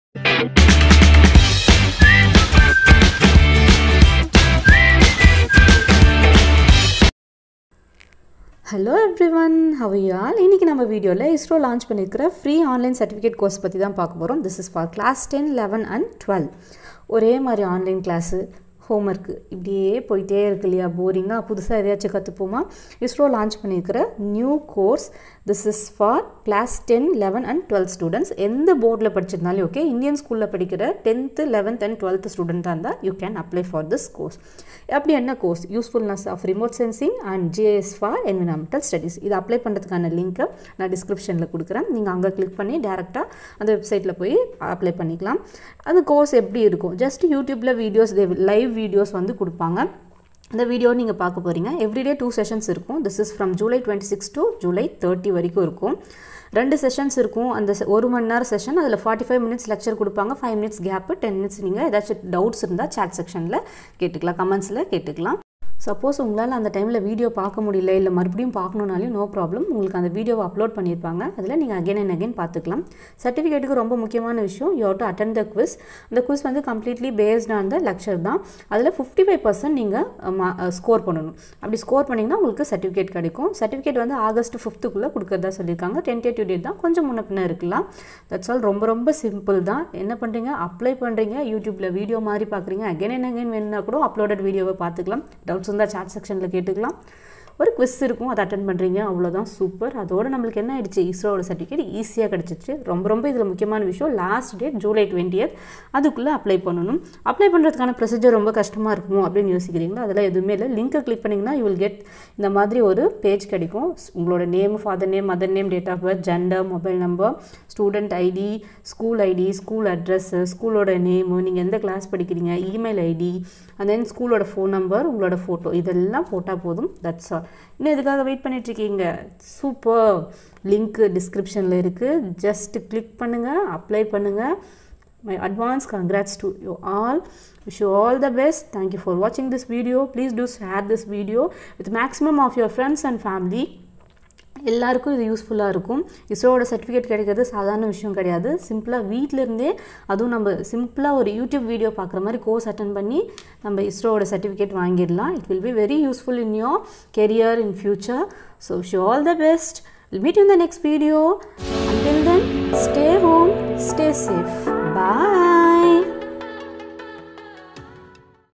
ISRO Free Online Certificate Course | Class X, XI & XII (All board) | Apply Now | Last date 👉July 20
🔥"ISRO" Free Online Certificate Course for Class X, XI & XII Students

🔥Online summer school on
Usefulness of Remote Sensing & GIS for Environmental Studies
(Course No. 101)

🔥Last Date 👉 20 July 2021

🔥The lecture will be streamed live at IIRS YouTube channel 👉